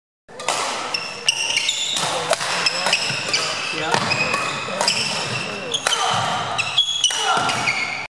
Badminton's Power Game
At the USA Badminton Adult Nationals, Nicolas Waller (near court) and Shawn Whong slug it out in their men's singles match.